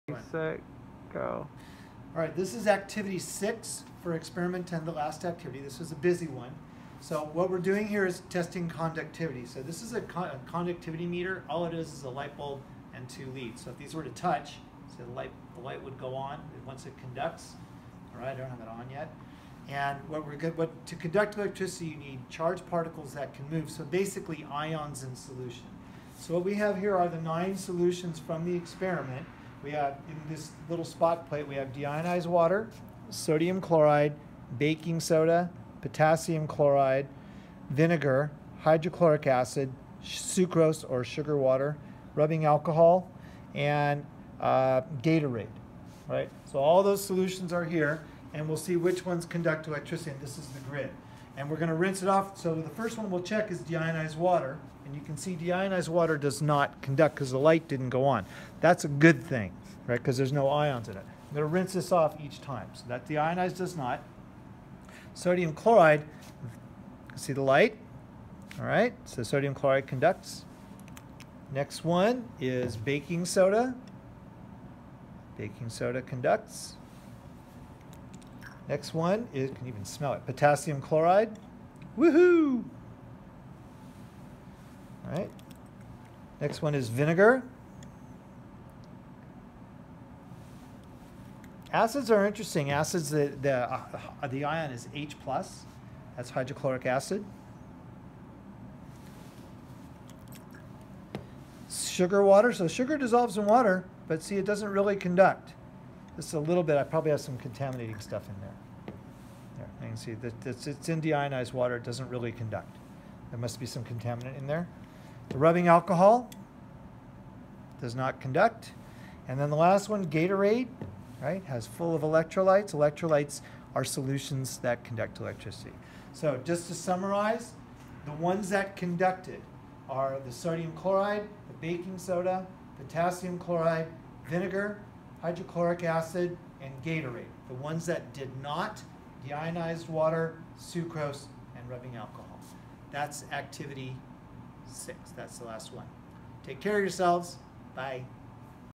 fremland111exp10act6
Mesa College Chemistry 111L Experiment 10: Water and its properties: Activity 6: Conductivity